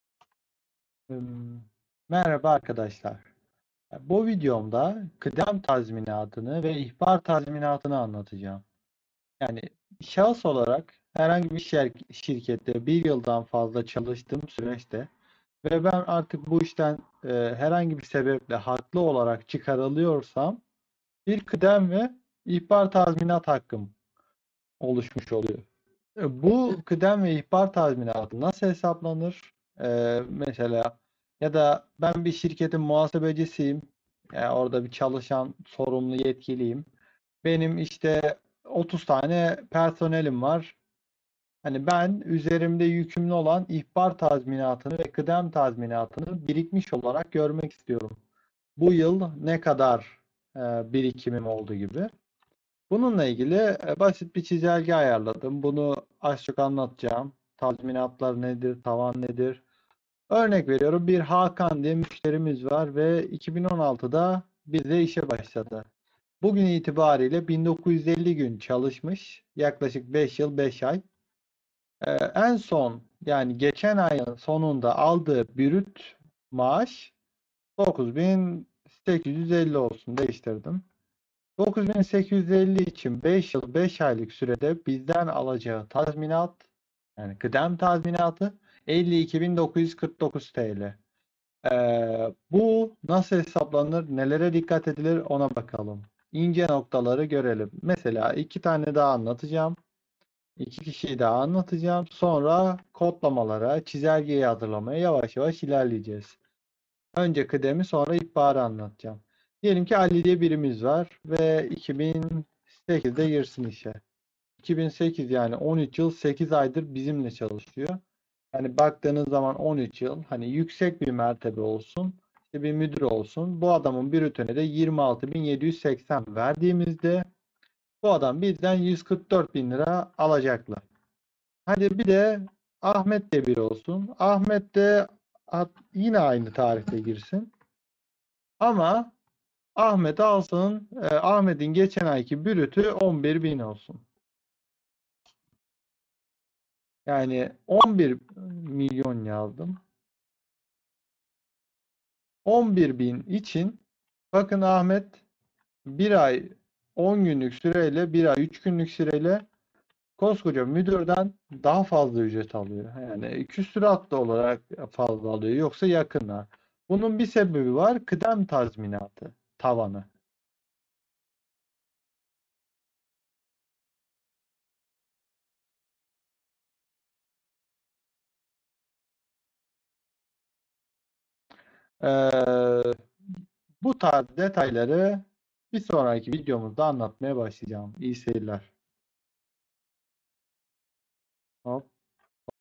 KIDEM TAZMİNATI HESAPLAMA 2022 Excel ile Tanıtım ve Ön Bilgi
Merhaba Arkadaşlar,
Videomuzda Şirketimiz personellerinin üzerimizde oluşturduğu yüklerden biri olan kıdem tazminatı nasıl hesaplanır.
Nelere dikkat edilmeli bunları anlatıyoruz.